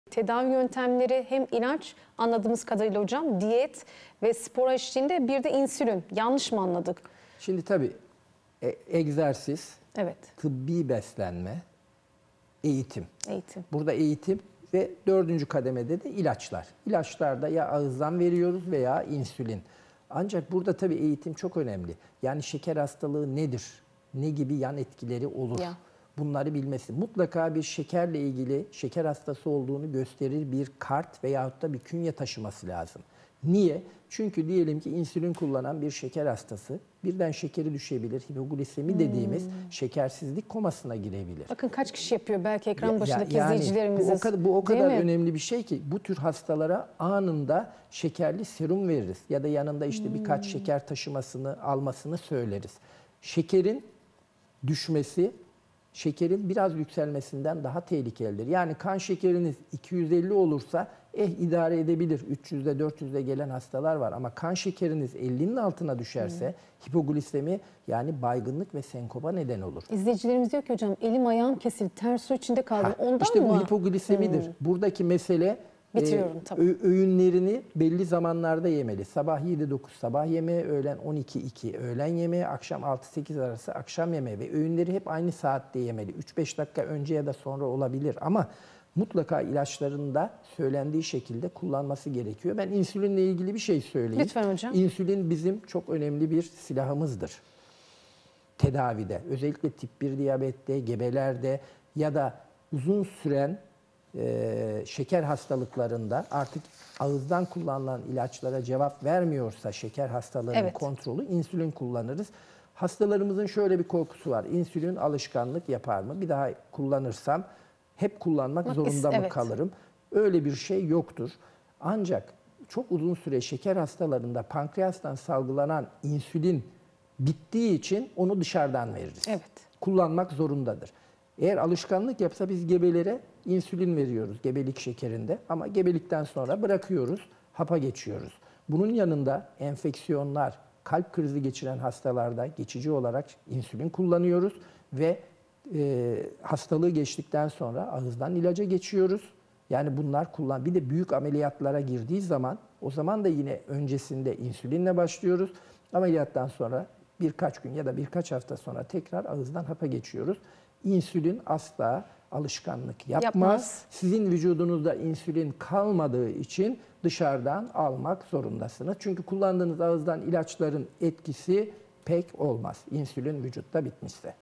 İç Hastalıkları Uzmanı Uzm. Dr. İsmail Çelik '' Şeker Hastalığı '' anlatıyor
Avrasya Hastanesi İç Hastalıkları Uzmanı Uzm. Dr. İsmail Çelik '' Şeker Hastalığı  '' hakkında detayı bilgiler veriyor